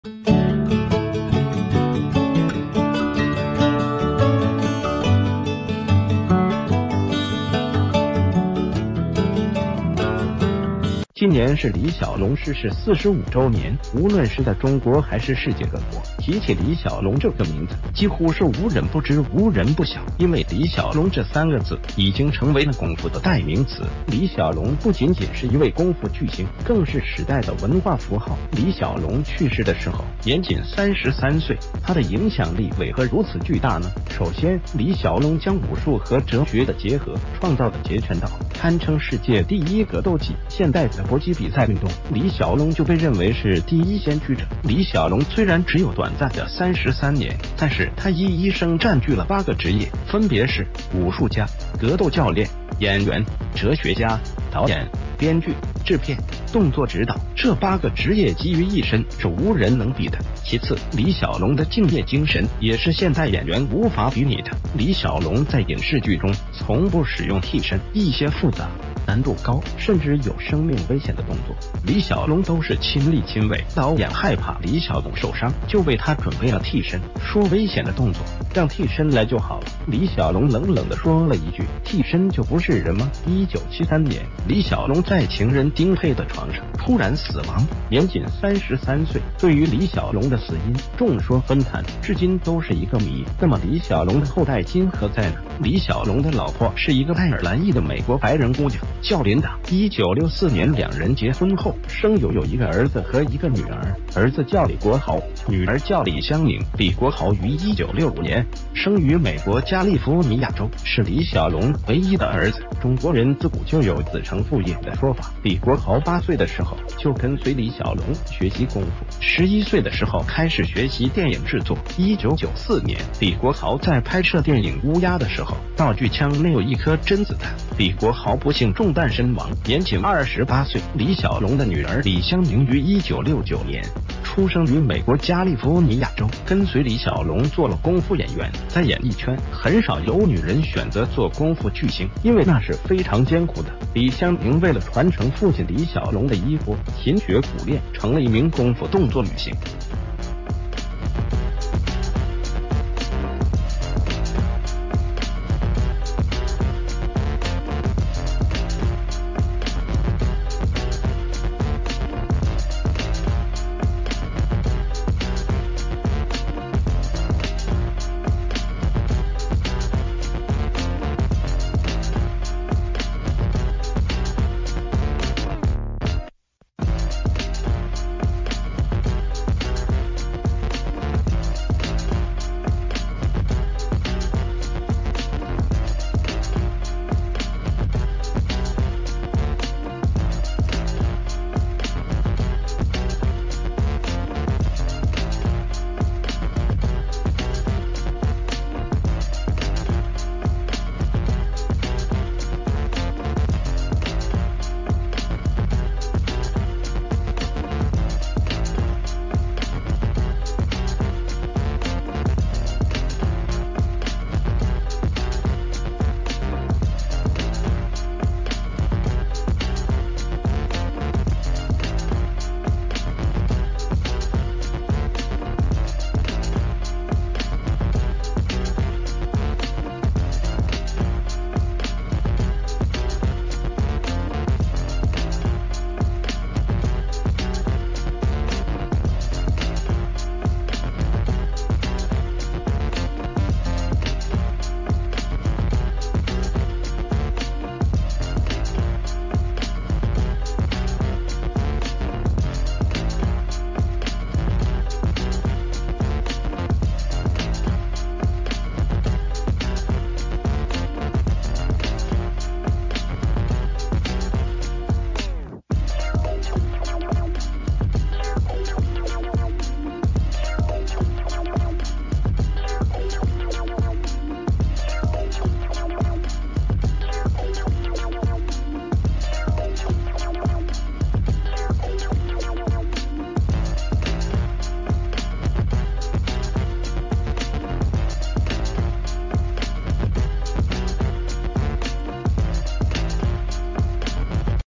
李小龍活了33歲，后代今何在？兒子活了28歲，女兒成動作女星
今年是李小龍逝世45周年，無論是在中國還是世界各國，提起李小龍這個名字，幾乎是無人不知無人不曉。因為李小龍這三個字，已經成為了功夫的代名詞。李小龍不僅僅是一位功夫巨星，更是時代的文化符號。李小龍去世的時候，年僅33歲，他的影響力為何如此巨大呢？
    首先，李小龍將武術和哲學的結合，創造的截拳道，堪稱世界第一格斗技，現代的搏擊比賽運動，李小龍就被認為是第一先驅者。李小龍雖然只有短暫的33年，但是他一一生占據了8個職業，分別是：武術家、格斗教練、演員、哲學家、導演、編劇、制片、動作指導。這8個職業集于一身，是無人能比的。
    其次，李小龍的敬業精神，也是現代演員無法比擬的。李小龍在影視劇中，從不使用替身。一些復雜、難度高，甚至有生命危險的動作，李小龍都是親力親為。導演害怕李小龍受傷，就為他準備了替身，說危險的動作，讓替身來就好了。李小龍冷冷地說了一句：“替身就不是人嗎？”
    1973年，李小龍在情人丁佩的床上，突然死亡，年僅33歲。對于李小龍的死因，眾說紛壇，至今都是一個謎。那么李小龍的后代今何在呢？
    李小龍的老婆是一個愛爾蘭裔的美國白人姑娘，叫琳達。1964年兩人結婚后，生有有一個兒子和一個女兒。兒子叫李國豪，女兒叫李香凝。
    李國豪于1965年生于美國加利福尼亞州，是李小龍唯一的兒子。中國人自古就有“子承父業”的說法，李國豪8歲的時候，就跟隨李小龍學習功夫，11歲的時候開始學習電影制作。1994年，李國豪在拍攝電影《烏鴉》的時候，道具槍內有一顆真子彈，李國豪不幸中彈身亡，年僅28歲。
    李小龍的女兒李香凝于1969年出生于美國加利福尼亞州，跟隨李小龍做了功夫演員。在演藝圈，很少有女人選擇做功夫巨星，因為那是非常艱苦的。李香凝為了傳承父親李小龍的衣缽，勤學苦練，成了一名功夫動作女星。李香凝的眼神有著李小龍的感覺，她在凝視對手的一剎那，仿佛李小龍再現。
    對此大家有什么想說的呢？